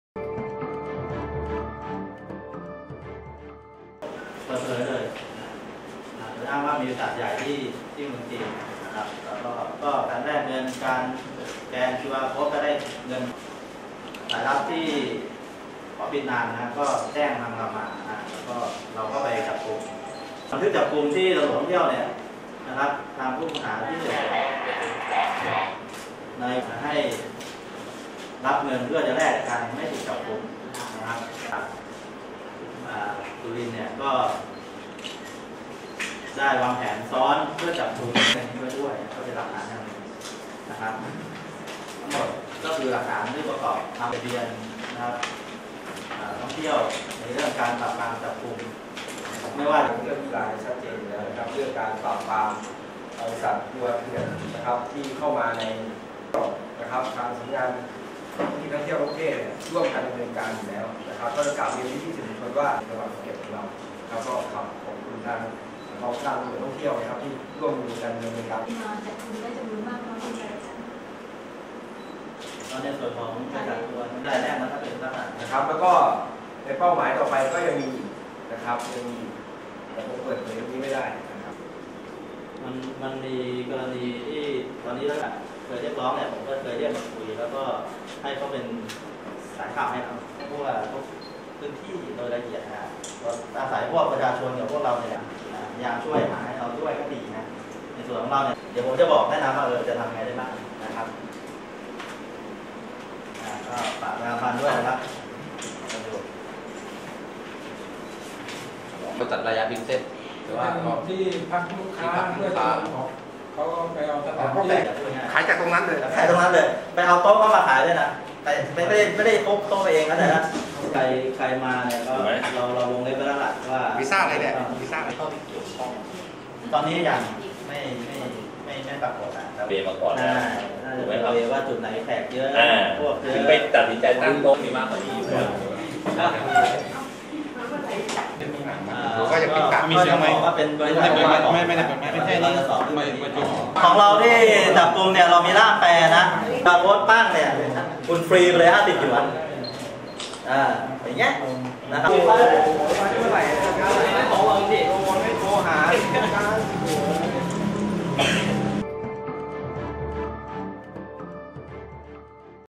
ADMTVจับ2ชาวจีนขายทัวร์เถื่อนพยายามติดสินบนจนท. 1เเสน
ตำรวจท่องเที่ยวร่วมนายทะเบียนธุรกิจท่องเที่ยวจับ 2 ชาวจีน เปิดขายทัวร์เถื่อนแก่นทท.เย้ยจนท.หลังถูกจับพยายามนำเงิน 1  แสนบาท ติดสินบน จึงโดนข้อหาหนัก